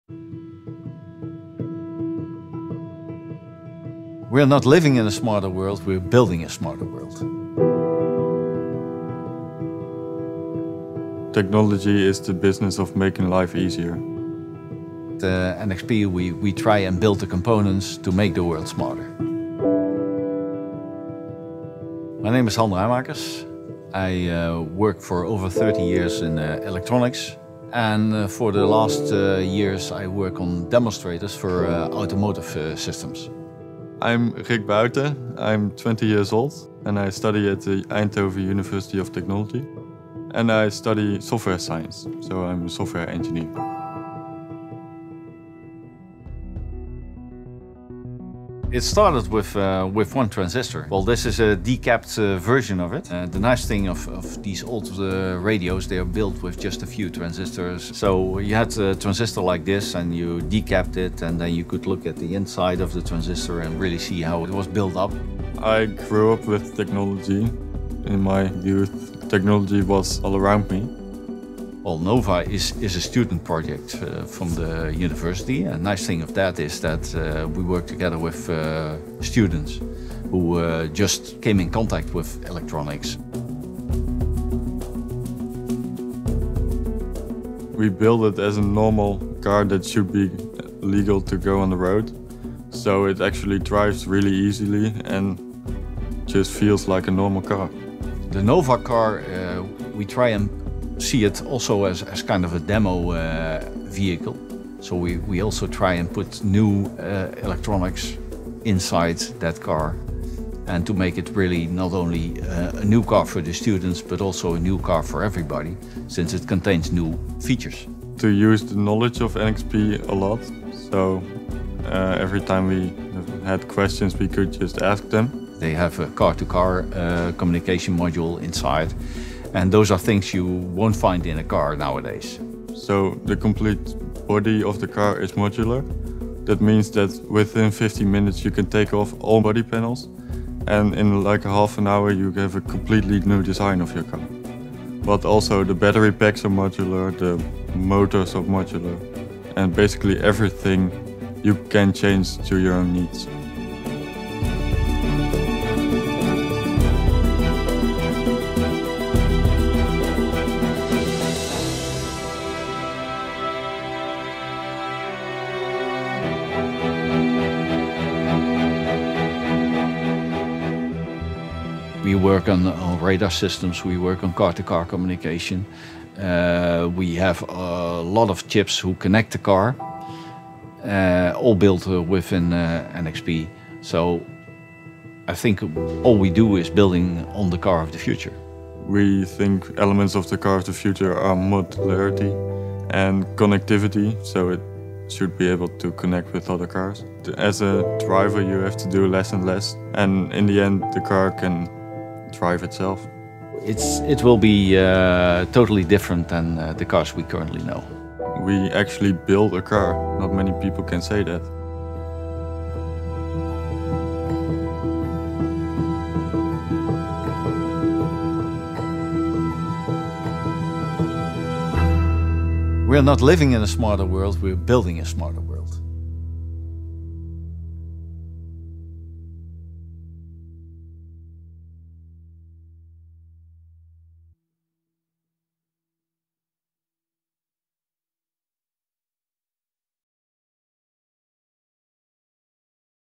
The Car Of The Future
We all know the world is getting smarter but who is it that’s building this new smarter world?

Almost everyone on the globe has their part to play in developing this world, but in particular dedicated and passionate engineers, thinkers and innovators are creating the smart technology solutions to the critical issues of today and tomorrow.

Just one of these powerful innovations is “Nova” the latest prototype modular electric vehicle from students at the Eindhoven University of Technology. Designed for the road, the one of a kind car demonstrates the latest in connected technology as well as an innovative, completely unique, modular design.

The project is the result of close collaboration between students at the university and NXP Semiconductors. The students built the car using NXP chip technology and relied on experienced NXP automotive engineers to advise them along the way.